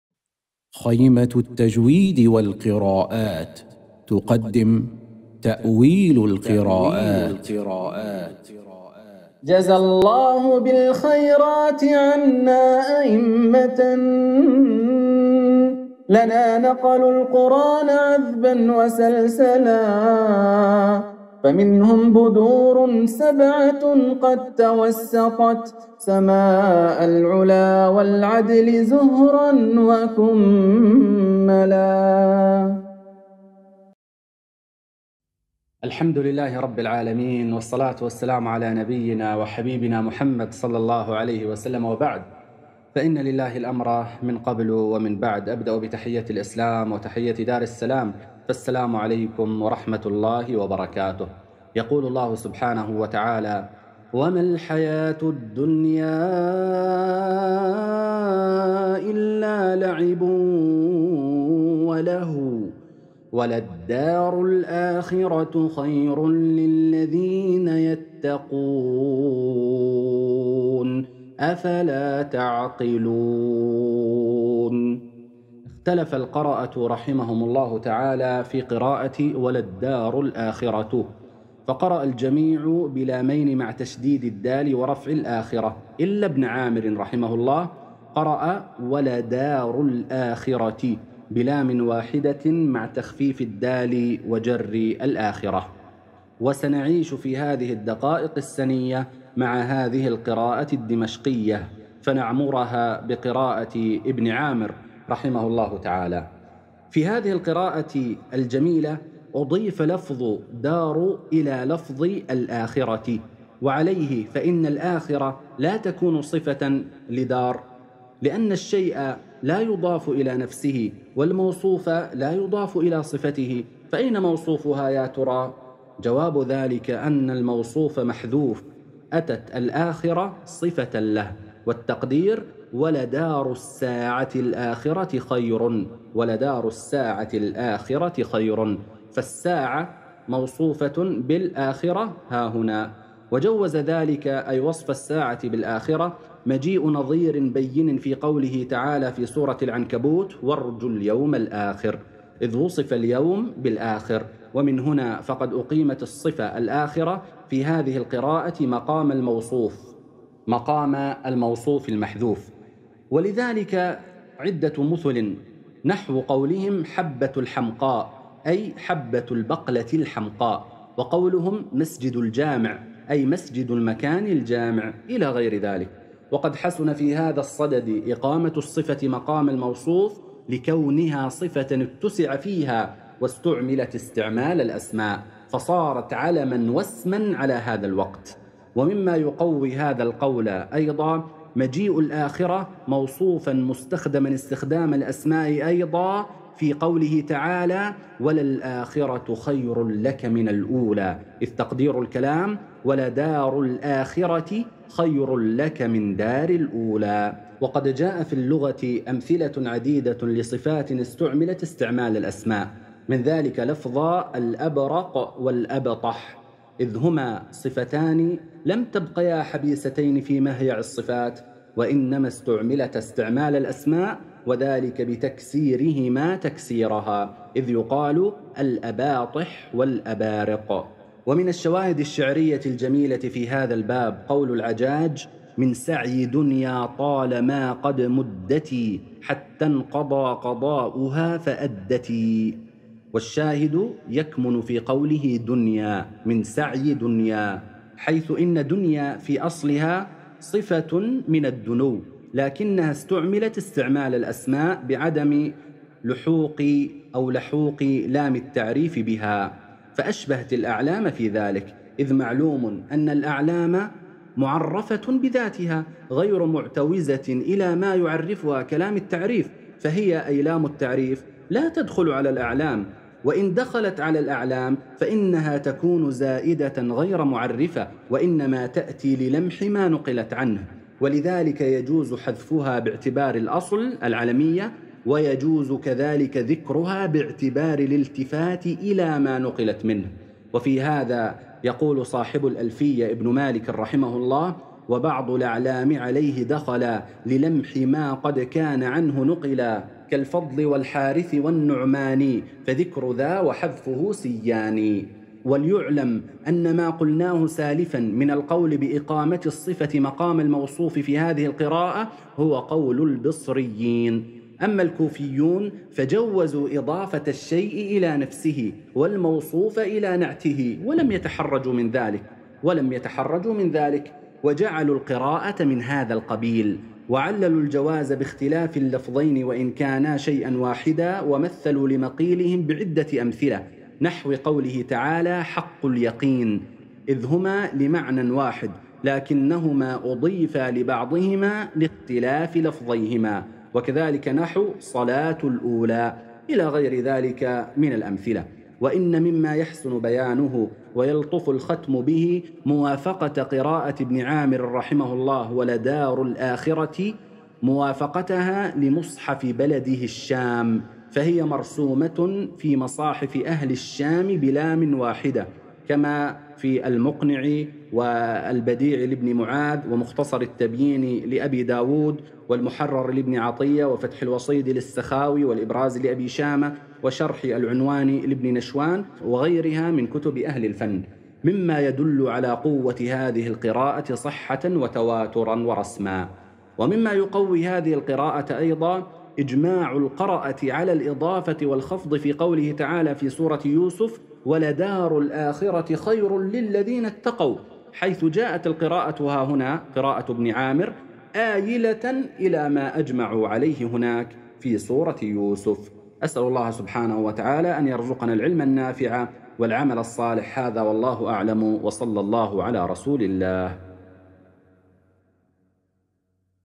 تأويل القراءات || الحلقة التاسعة || تأويل قراءة ابن عامر ( ولدار الآخرة ) د سعيد النمارنة.
تعليم أحكام التلاوة بطريقة نور البيان
جمع القراءات العشر
شرح متون القراءات